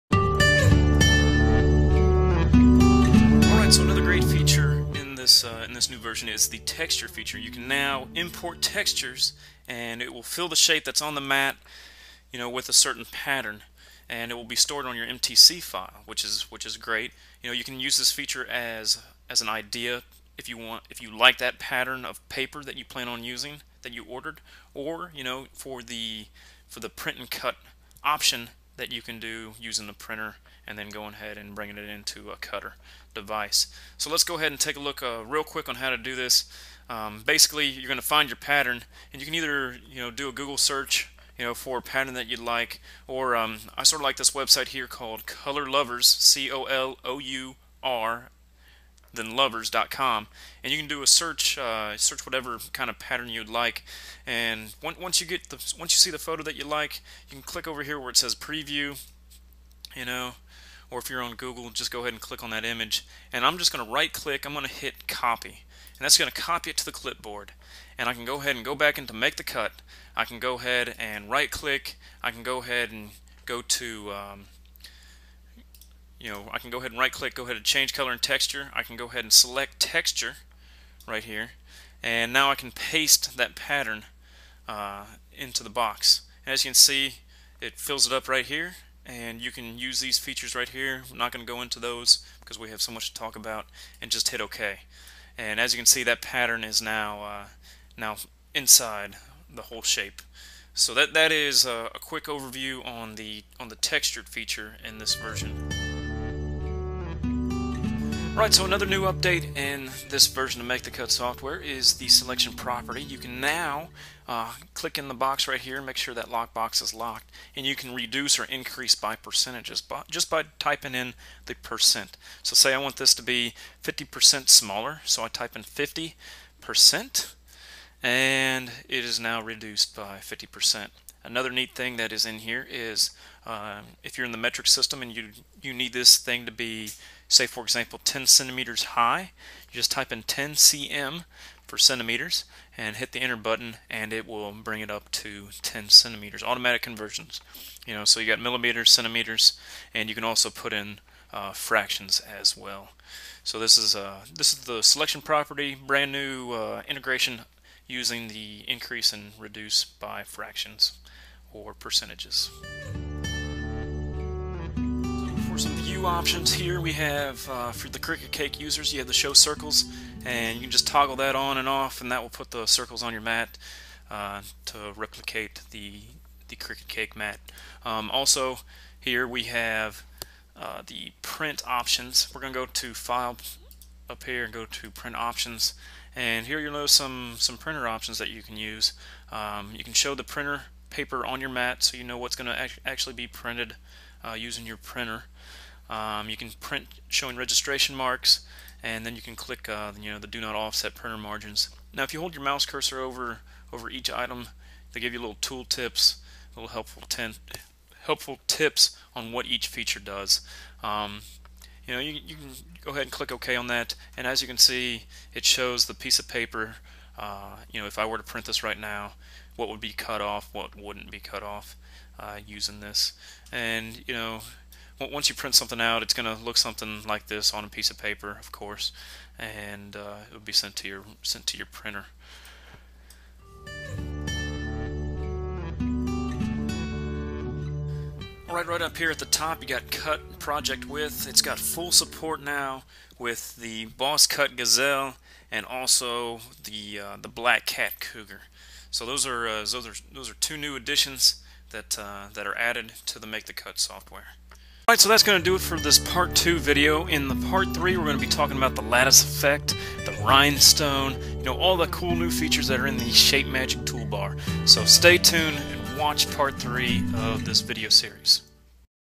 Video 2: Make the Cut Overview 3.0
Part 2 of the Make the Cut Software. Version 3.0 Beta was released today and we're putting up a video tutorial showing you what these new features are.